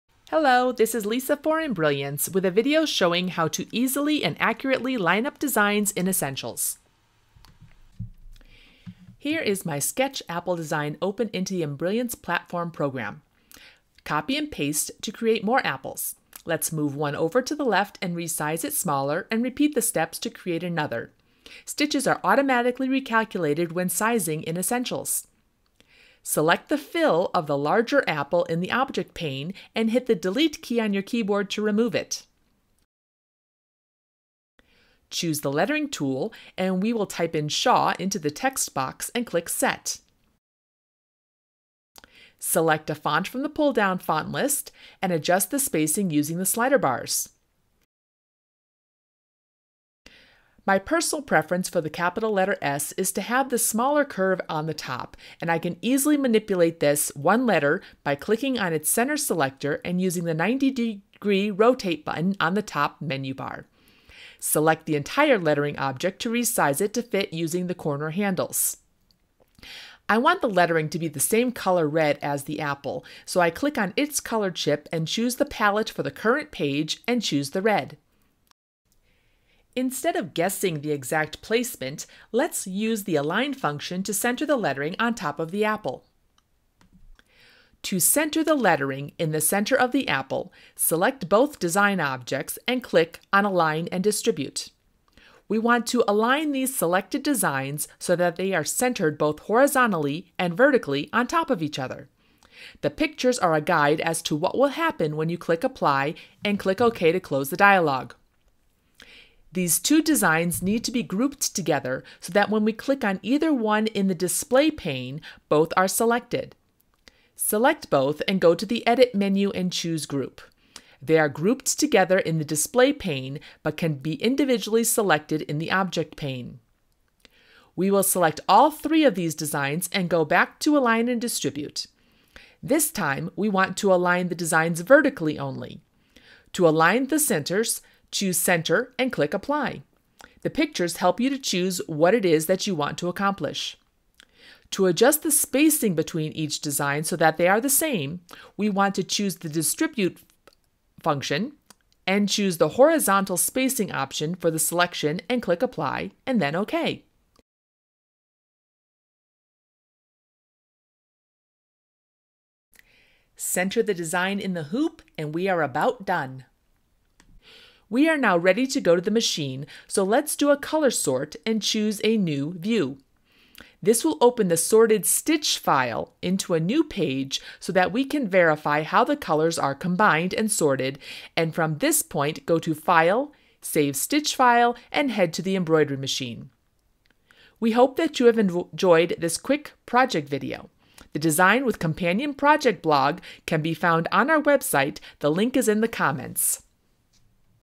How to Align & Distribute Designs in Essentials
This is a companion video to the October 2018 project blog post that provides the free design and PDF instructions for creating this layout.  The PDF and this video shows how to use the easy but powerful tools of Embrilliance Essentials.  Although the program shown has other modules installed, the functions used in this video are part of Essentials.

The design we are working with in this video was digitized in Embrilliance StitchArtist and opened as a BE working file.  More information on the design can be found in the blog post linked below.

The Align function allows you to position selected design elements in an uniform manner - for example to exactly place the bottom of every design at the same horizontal location.

The Distribute function allows you to determine how you would like your selected designs to be positioned in relation to one another OR in relation to the hoop either horizontally, vertically or both.

The Align and Distribute function is for working with multiple designs.  For information on how to line up, adjust spacing, create vertical or circular layouts with lettering objects, check out the videos in our Lettering Playlist.  The lettering quick styles give you even easier command and control over design elements created with the lettering tool!

Other techniques shown in this video are how to change the color of a selection - for example the added lettering - to one that is already in use in the design. When combined with the color sort function (also part of Embrilliance Essentials) your stitch file will have fewer thread changes.  This can be a time saving feature at the machine!